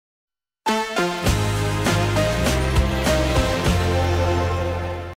Canal Cinco - Vinheta (2014-2015)
Antiga vinheta interprogramas do Canal Cinco, exibida de 21 de Abril de 2014 a 21 de Abril de 2015. O slogan utilizado na altura era «Nosso canal perto de todos».